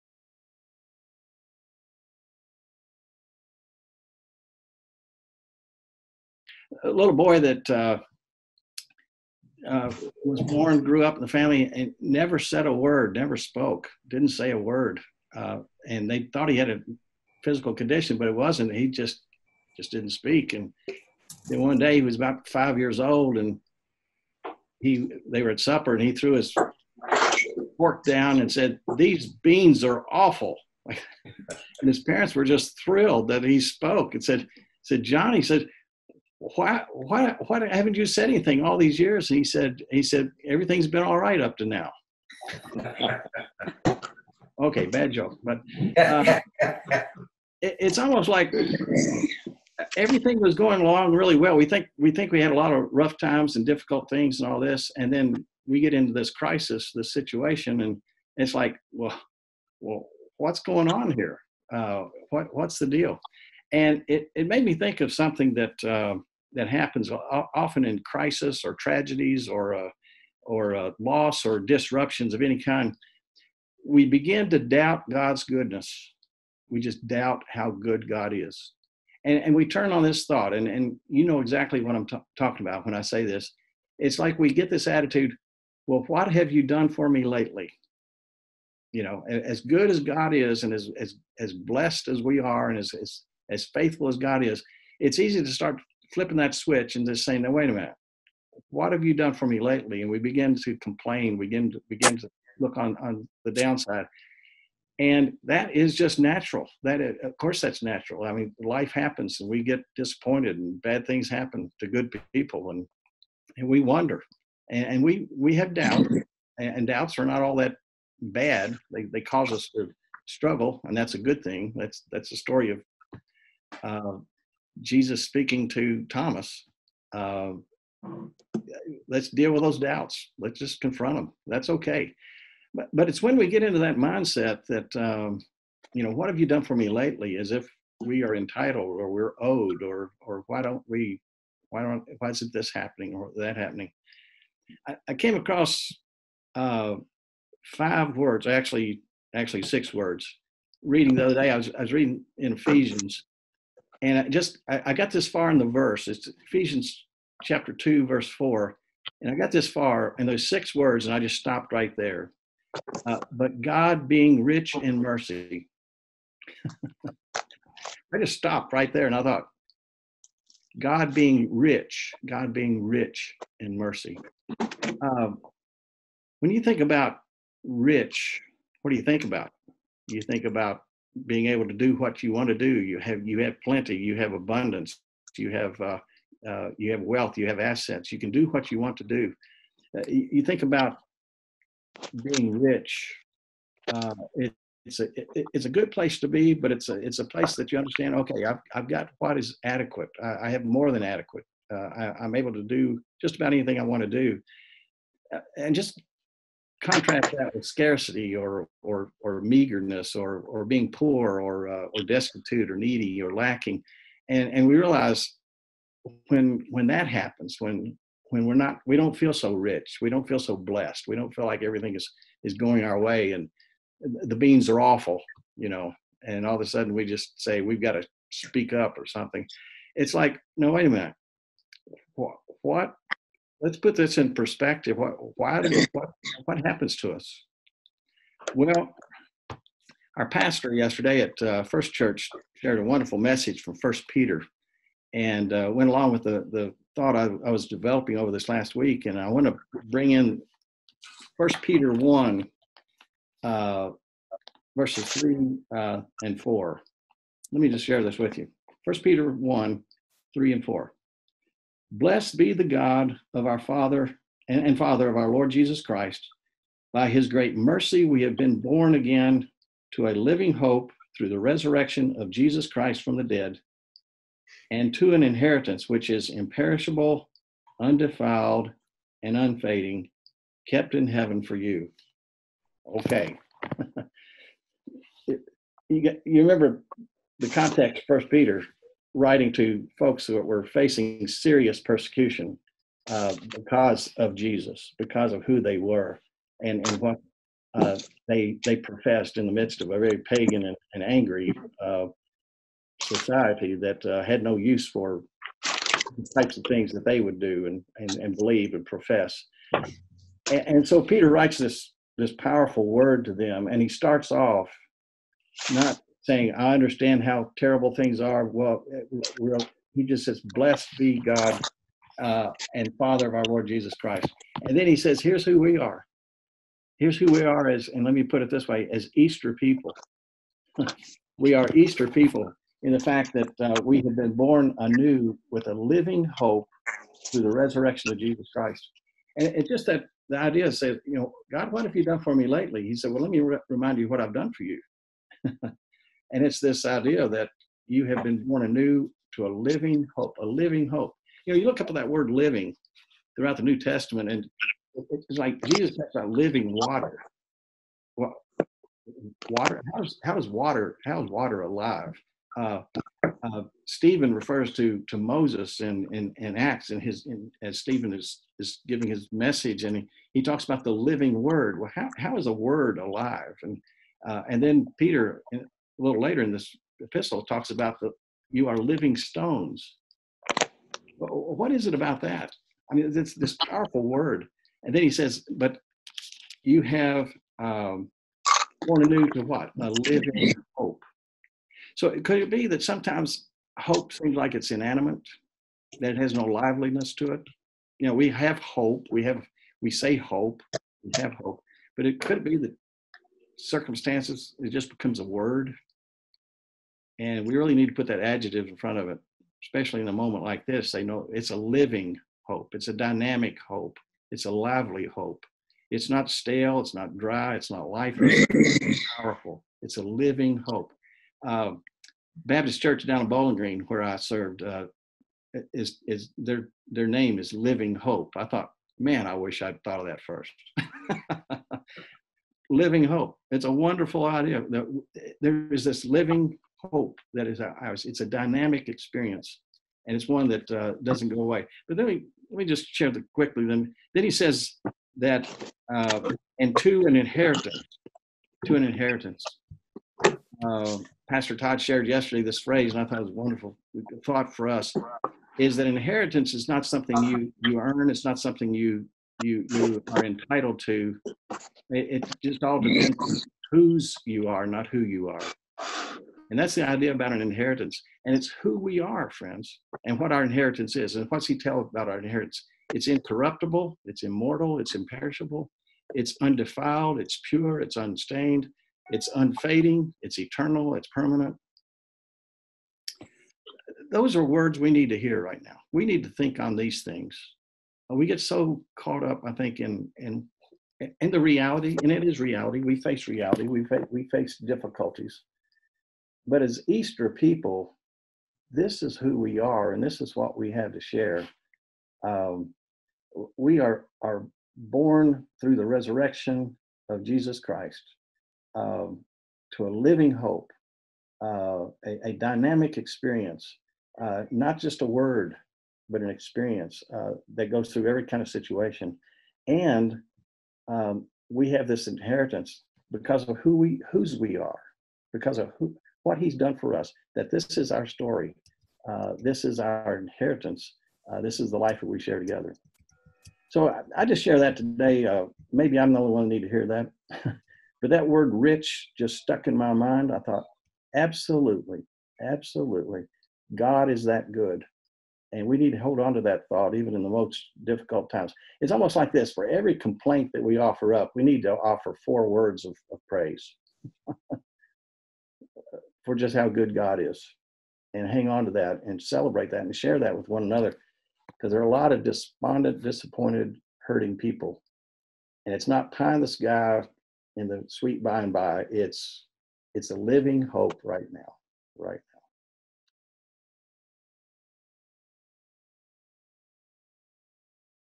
"God Is Our Living Hope" by Mike Powers
In times of crisis and uncertainty, we are often tempted to doubt God's goodness. We forget that, through Christ, he is our living hope.